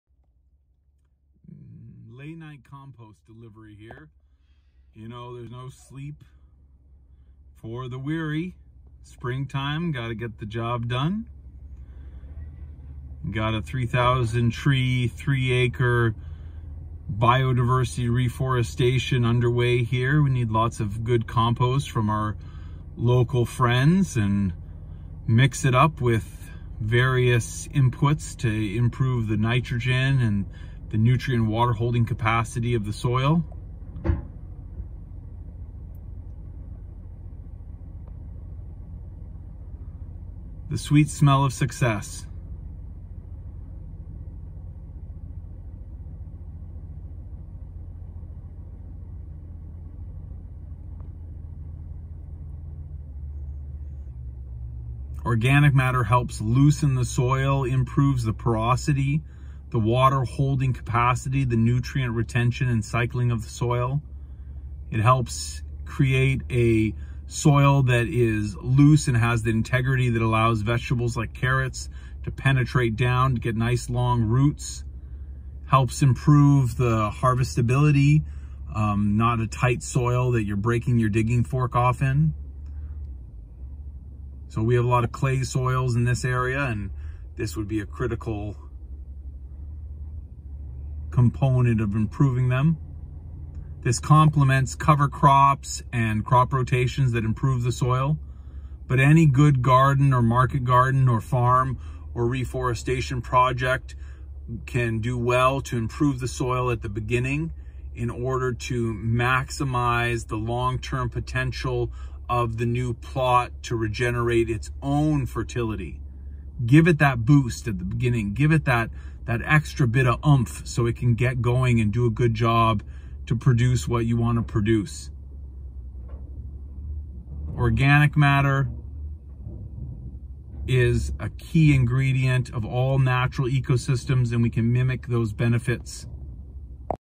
Late night compost delivery makes extra might garden!
Compost improves our soil: increasing water holding capacity, nutrient retention, and the ability of the soil to eventually regenerate its own fertility- improve your gardens from the get-go.